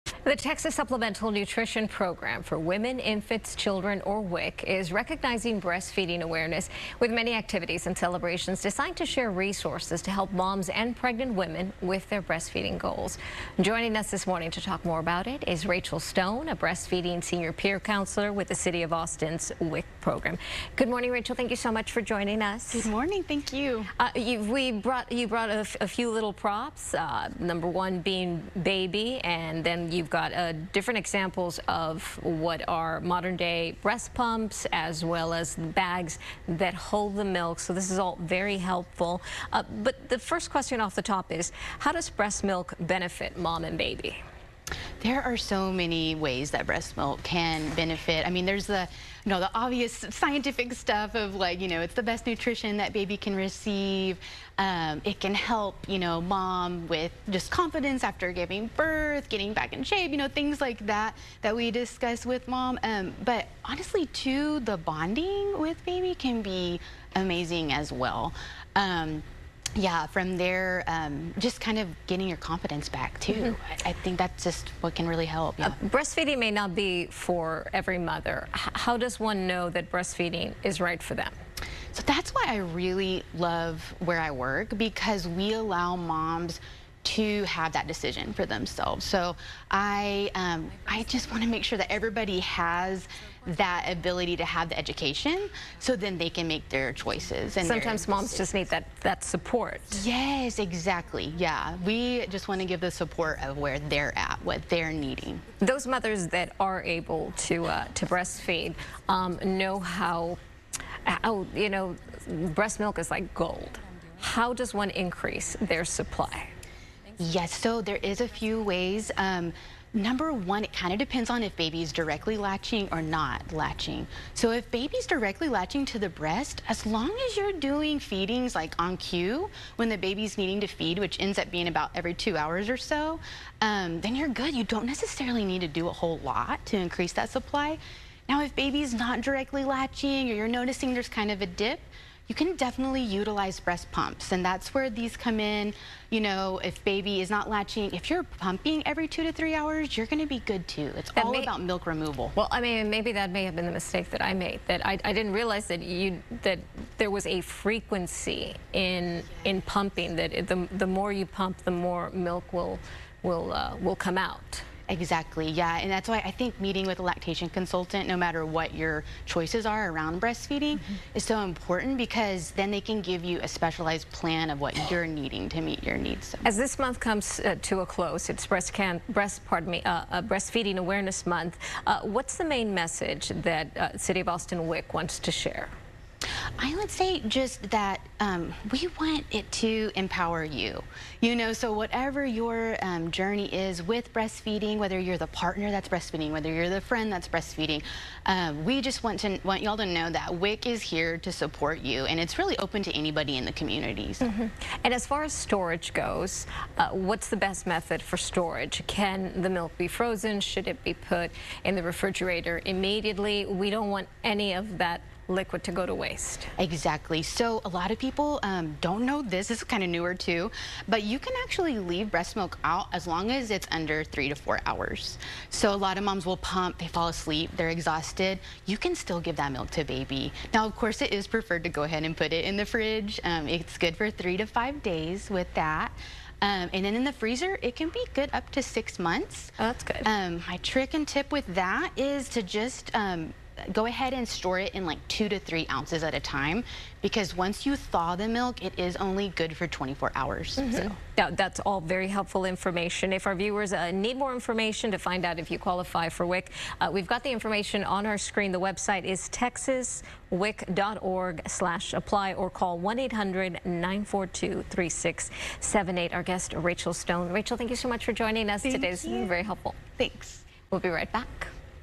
Breastfeeding resources for Texas women | FOX 7 Austin
August is National Breastfeeding Awareness Month and the Texas Supplemental Nutrition Program for Women, Infants, Children, or WIC, is recognizing the month with a host of activities designed to share resources with moms to help them with their breastfeeding goals.